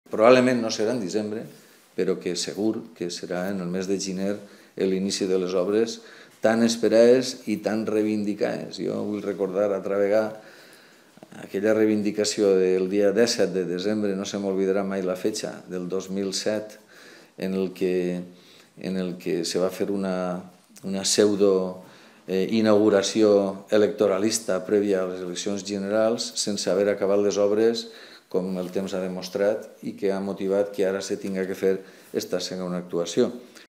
Costas adjudicará la regeneración de la playa de Benafelí en diciembre
La Dirección General de Sostenibilidad de la Costa y del Mar prevé adjudicar la regeneración ambiental y lucha contra la erosión en las playas de Benafelí, en Almassora, el próximo diciembre. “Siete años después de que de forma pública reclamáramos que se ejecutara esta obra tan necesaria”. El alcalde de Almassora, Vicente Casanova, ha hecho estas manifestaciones en su comparecencia semanal ante los medios de comunicación manteniendo que “durante cinco años quienes hicieron oídos sordos a nuestras demandas del 17 de diciembre de 2007, mantuvieron en una absoluta parálisis esta actuación”.